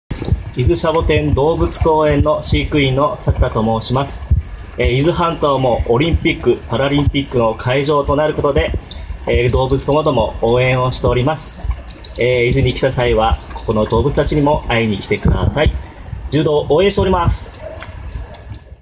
静岡新聞「私と五輪」作田武志さん
作田武志さん（４４）　伊東市　伊豆シャボテン動物公園飼育員
伊豆半島も会場になる東京五輪・パラリンピックを動物ともども応援しています。伊豆に来たら動物たちにも会いに来てください。